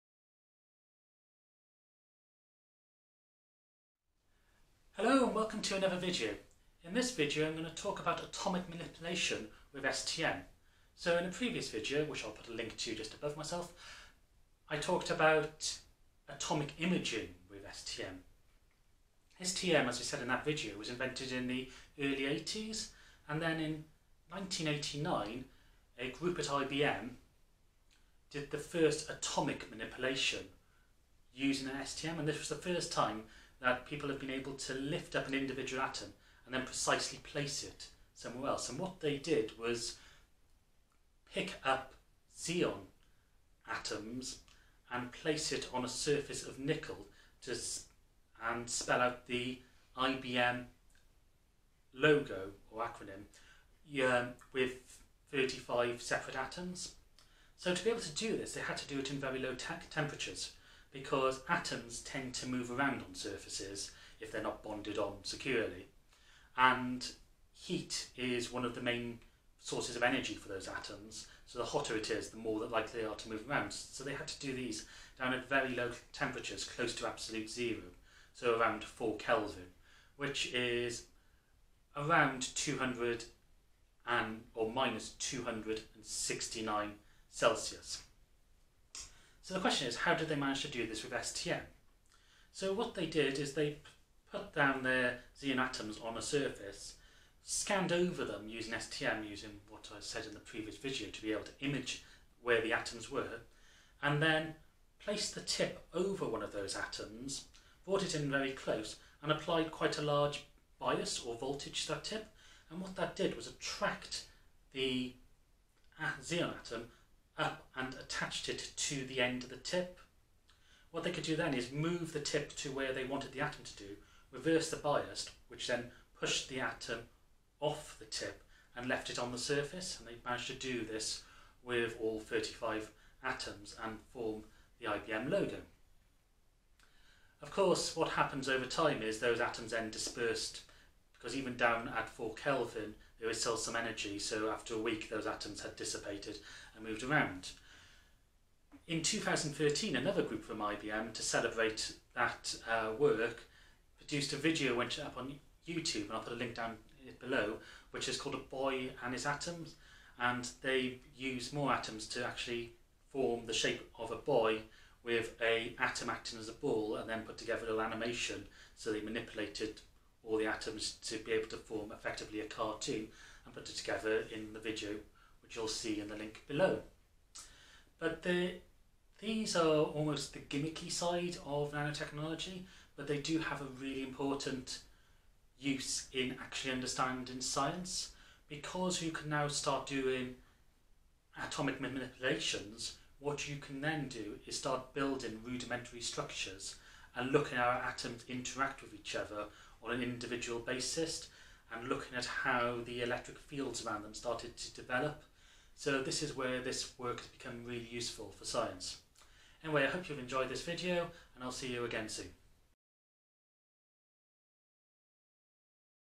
An Introduction to Atomic Manipulation with STM
This is a brief introduction how a Scanning Tunneling Microscope (STM) can move and manipulate atoms.